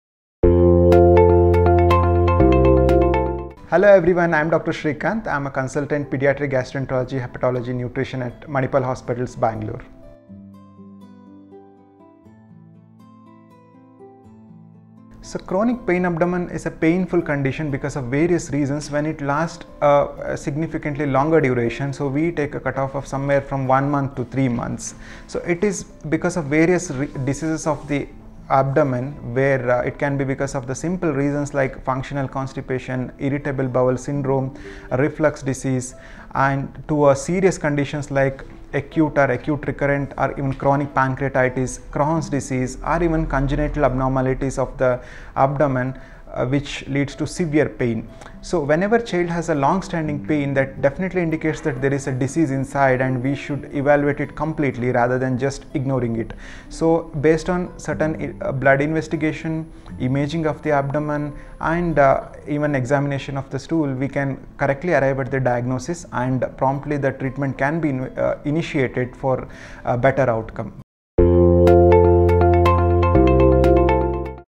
What is chronic pain in abdomen in children? - Dr. Srikant. K. P
Dr. Srikant. K. P explains What is chronic pain in abdomen in children.